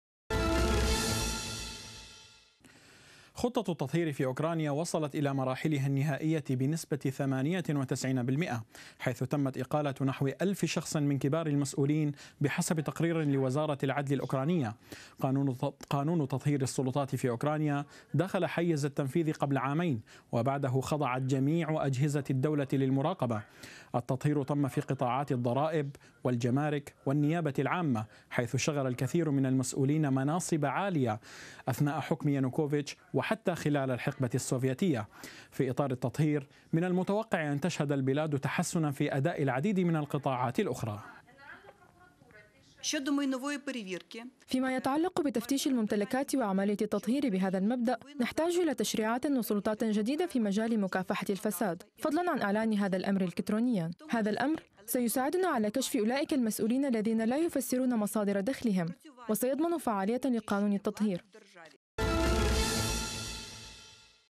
عامان على عملية "تطهير مؤسسات الدولة" في أوكرانيا
خطة التطهير في أوكرانيا وصلت إلى مراحلها النهائية بنسبة 98%، حيث تمت إقالة نحو ألف شخص من كبار المسؤولين، بحسب تقرير لوزارة العدل الأوكرانية.

قانون تطهير السلطات في أوكرانيا دخل حيز التنفيذ قبل عامين. وبعده خضعت جميع أجهزة الدولة للمراقبة.

التطهير تم في قطاعات الضرائب والجمارك والنيابة العامة، حيث شغل الكثير من المسؤولين مناصب عليا أثناء حكم يانوكوفيتش، وحتى خلال الحقبة السوفياتية.

في إطار التطهير، من المتوقع أن تشهد البلاد تحسنا في أداء العديد من القطاعات الأخرى.